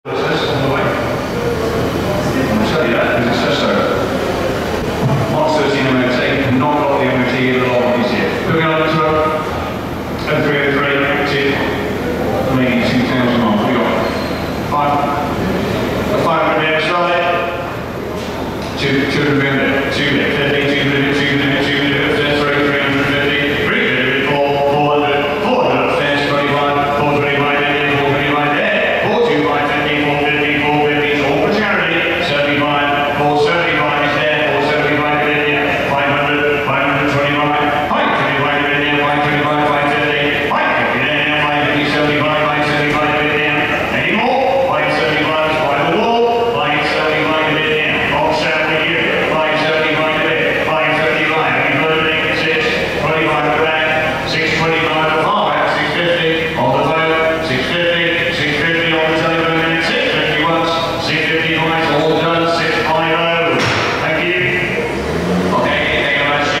BigBen5
BCA Blackbushe sold six cars today (31 October 2012) that had completed the 2012 Big Ben Rally to Barcelona to raise funds for BEN, the automotive industry charity.  All the money raised went direct to BEN and BCA additionally donated all the buyers fees.  This Vauxhall Astra 1.6 sold for £650